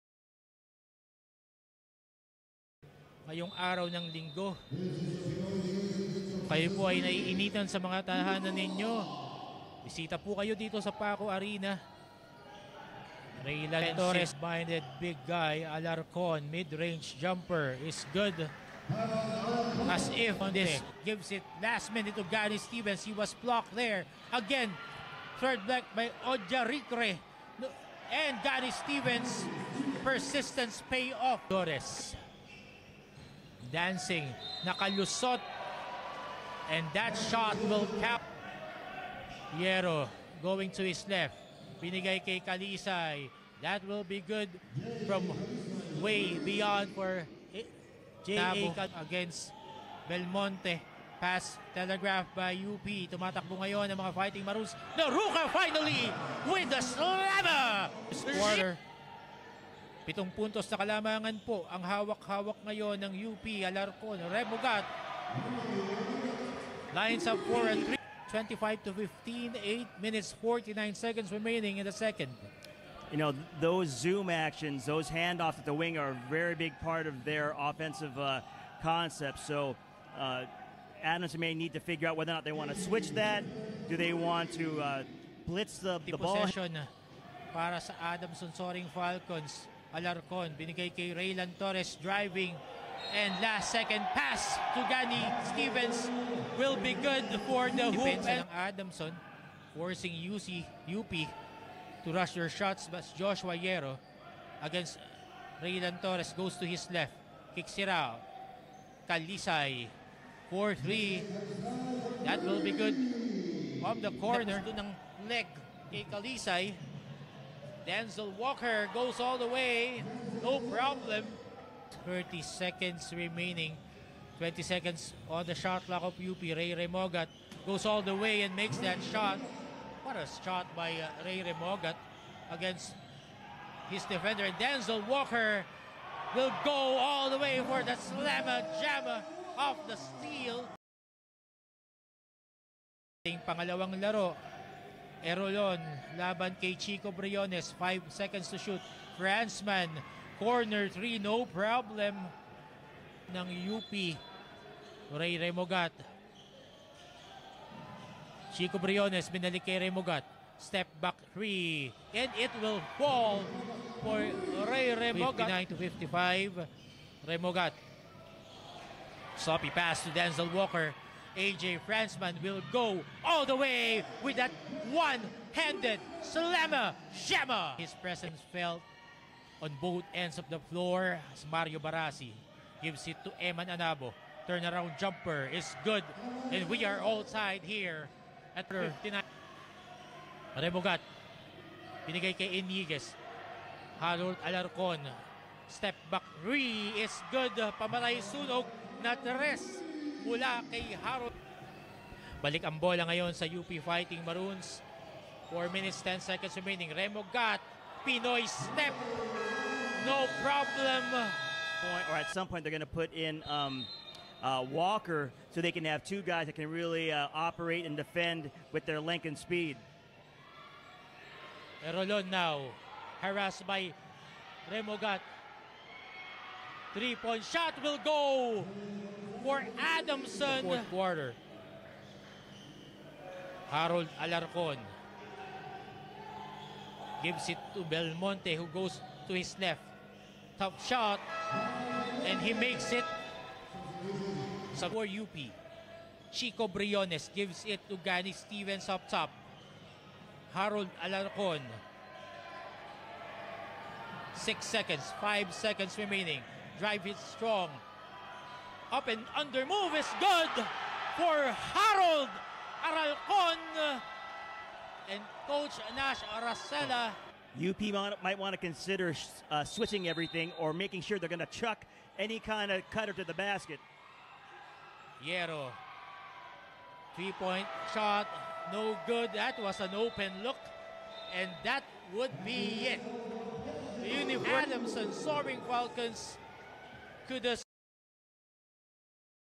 Game Highlights: UP Fighting Maroons vs. Adamson Soaring Falcons
The Maroons were taken to the deep ends by the Falcons in what turned out to be UP’s most competitive game this tournament. Harold Alarcon was a new age Paul Desiderio with a 2024 “atin to!” moment.